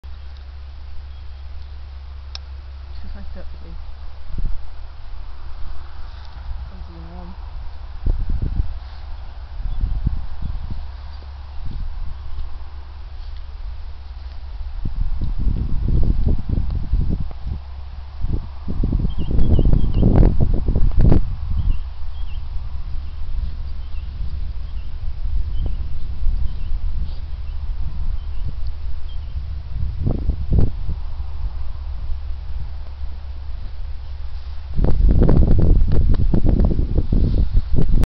Flee and Marcia rugging up
Marcia rugging up Flee on a cold day.